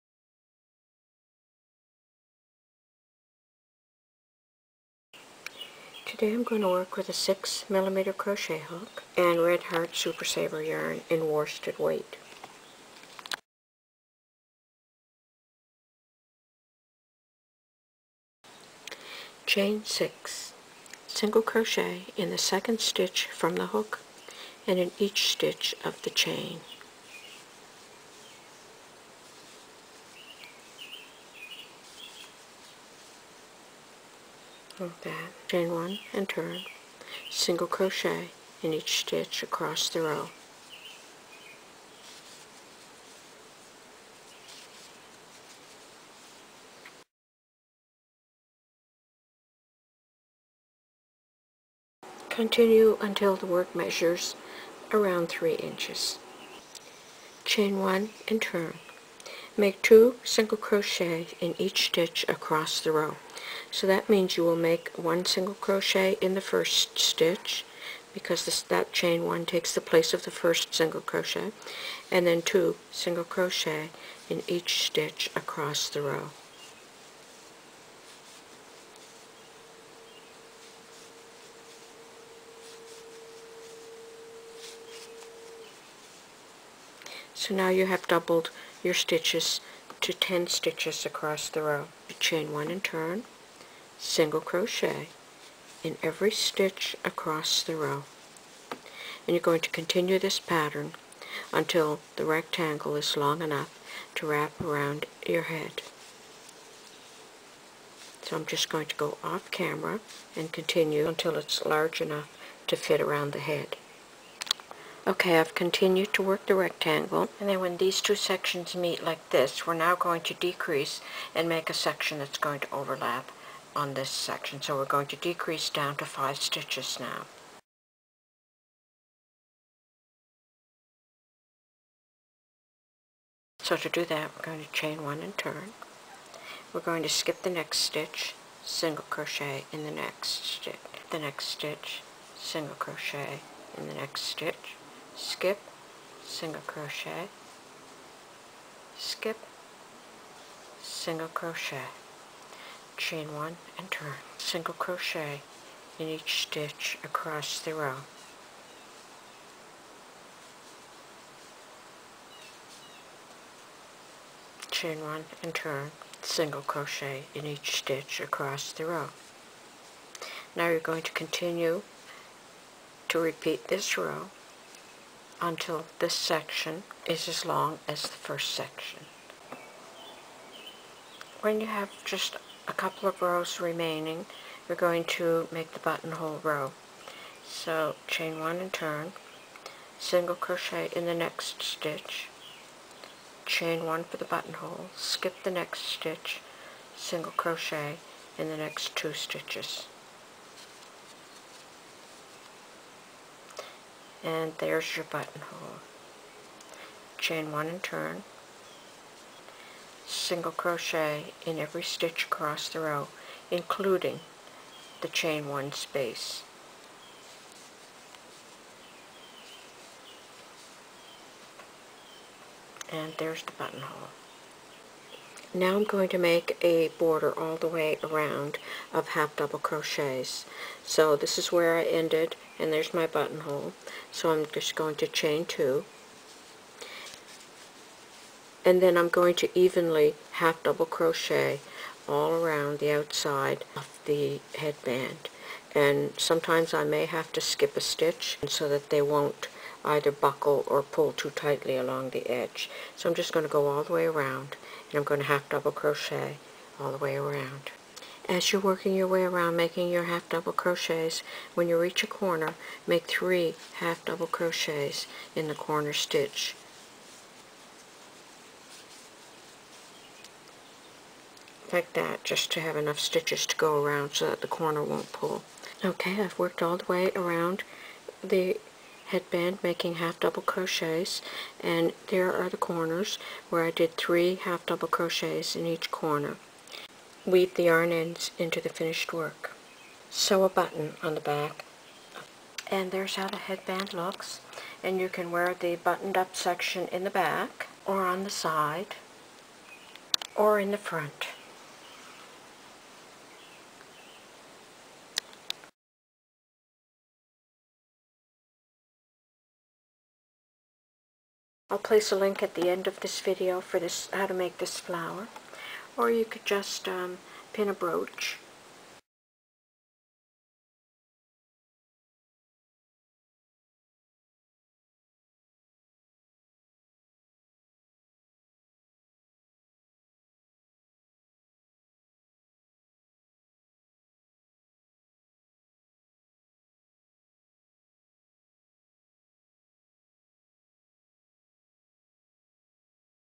HOW TO CROCHET A BUTTON TAB HEADBAND, toddler to adult
crochet pattern, headband, button up headband. How to crochet a headband.

MY SHOPS:
(new Hectanooga shop with only hats and scarves)

(Knitting, and sewing patterns)
(sewing, crochet, knitting patterns)

&index=98&feature=plpp_video